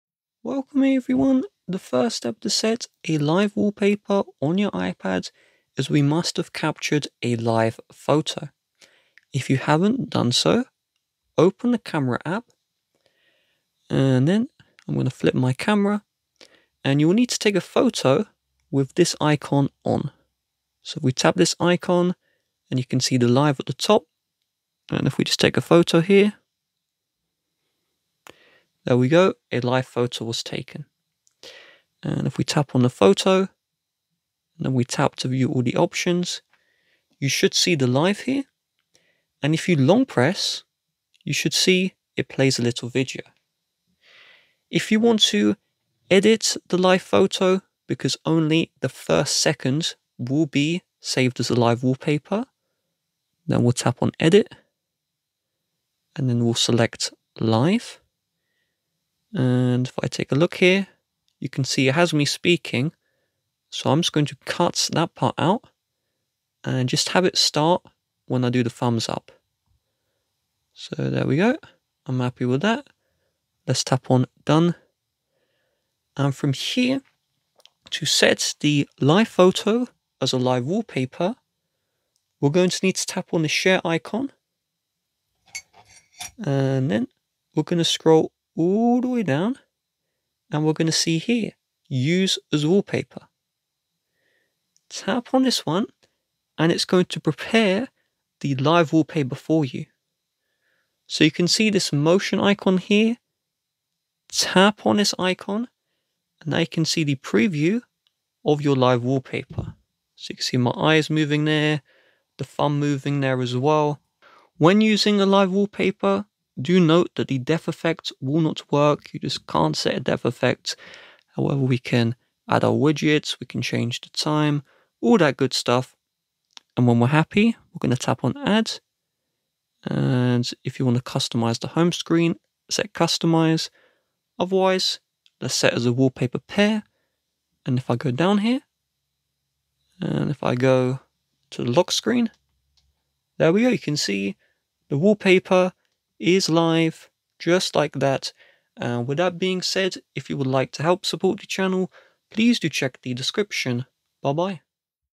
How To Set Live Wallpaper on iPad iPadOS 17
Learn how to set live wallpaper on iPad. Thanks to the iPadOS 17 software update, and all future iPadOS versions, live wallpapers is finally a feature available on all iPads.